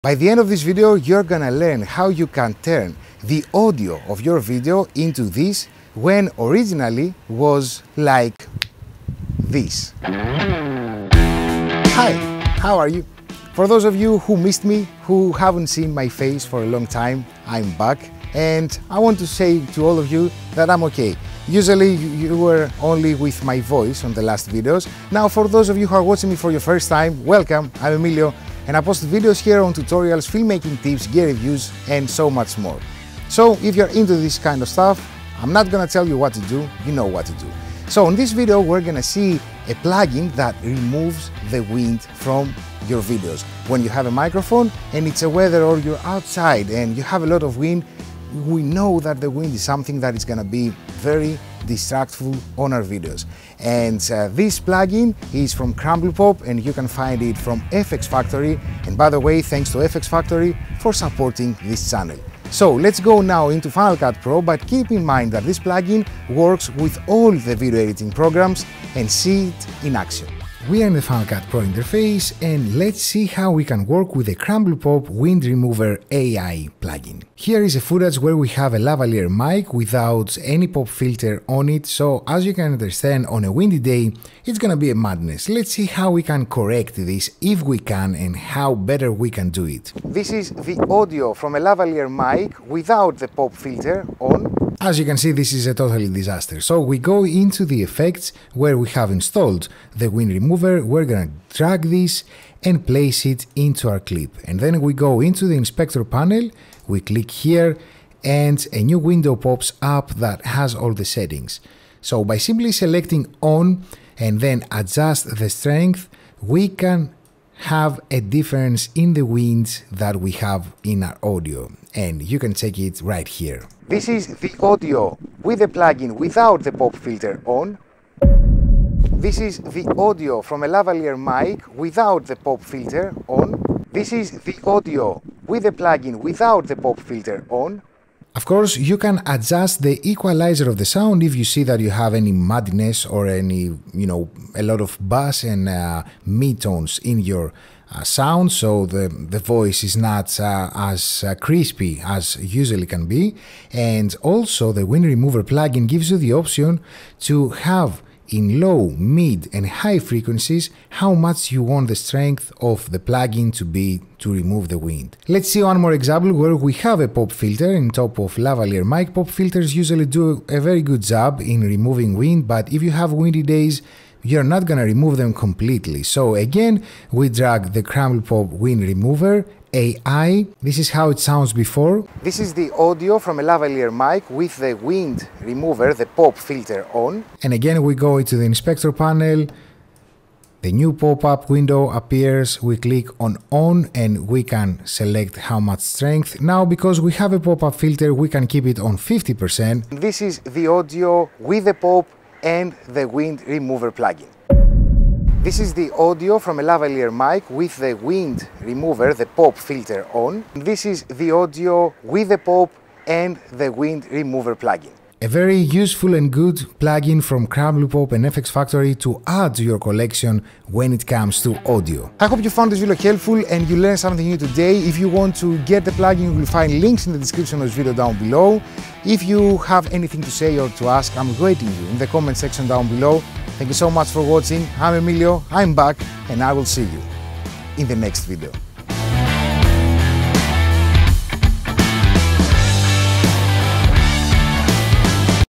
How To Remove Wind Noise (Final Cut Pro Tutorial)
In this Final Cut Pro Tutorial we are taking a look on how you can easily remove the so much NOT wanted wind noise from the audio of your videos. It is worth mentioning here that if you have the option to get a pop filter, or wind protection for your microphone that works the best. So consider this way of removing the noise just as an additional step or when you try to save the audio from a video.

TIMESTAMPS

0:00 - Intro
01:10 - Tutorial
03:42 - Outro